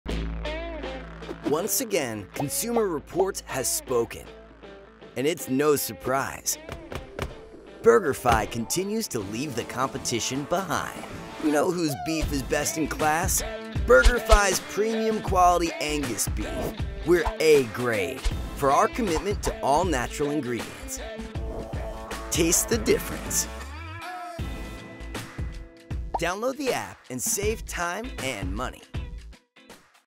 Our Premium Beef Continues to Leave the Competition Behind!
BurgerFi is committed to providing the best burger experience.  That’s why we serve chef-created, fresh food in an eco-friendly environment. We use 100% natural American Angus Beef with no steroids, antibiotics, or growth hormones.